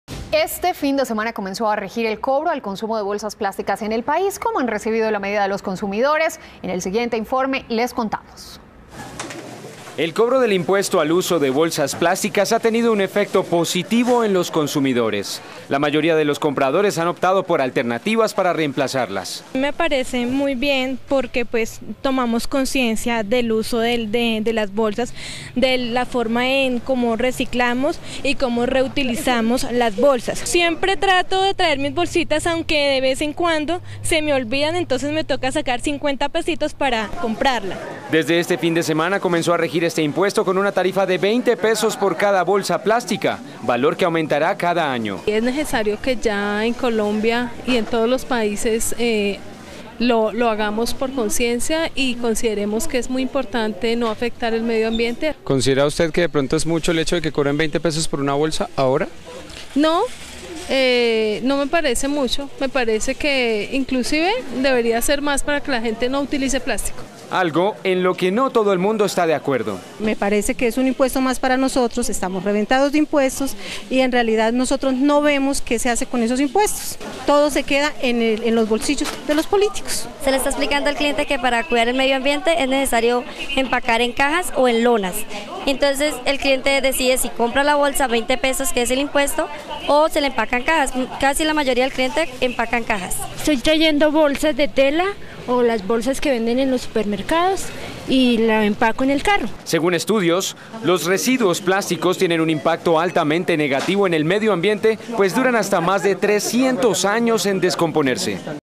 Colombianos, juiciosos con el ahorro de bolsas plásticas
Este fin de semana comenzó a regir el cobro por el consumo de talegas para el mercado. La mayoría de los consumidores ya han optado por varias alternativas."Me parece muy bien porque tomamos conciencia del uso de las bolsas, de la forma en que reciclamos y como reutilizamos las bolsas", opinó Dayana Díaz, cliente de un almacén de cadena.Quien no lleve canasto ni bolsa, tendrá que pagar una tarifa de 20 pesos por cada una. Dicho valor aumentará cada año.Según estudios científicos, los residuos plásticos tienen un impacto altamente negativo en el medio ambiente. Esto, ya que pueden duran hasta más de 300 años en degradarse.